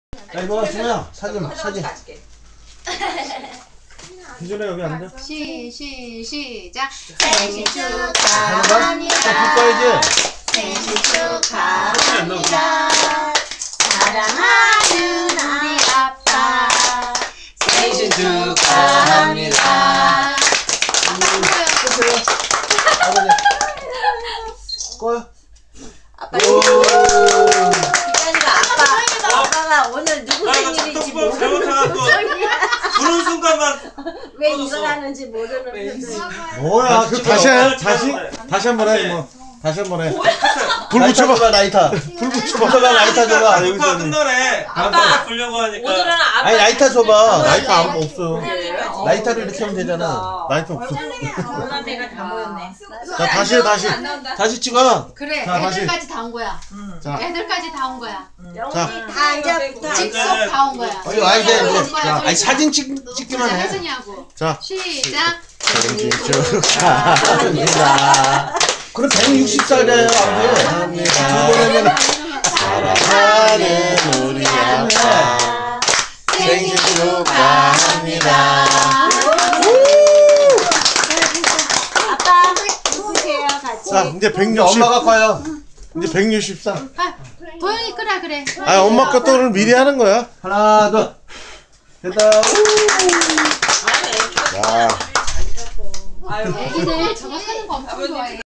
(김태웅영상)아버지 80세 생신잔치
가족모두 모여서 즐거운 식사시간